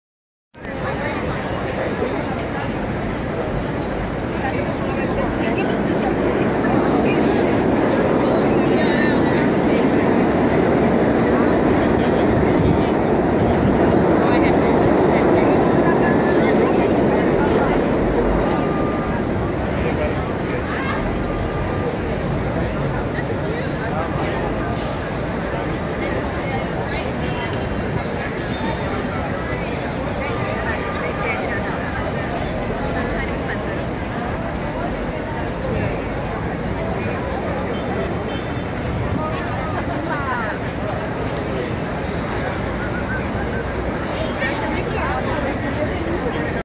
Times Square, Manhattan, New York City
panoramic video of our visit to Times Square, New York City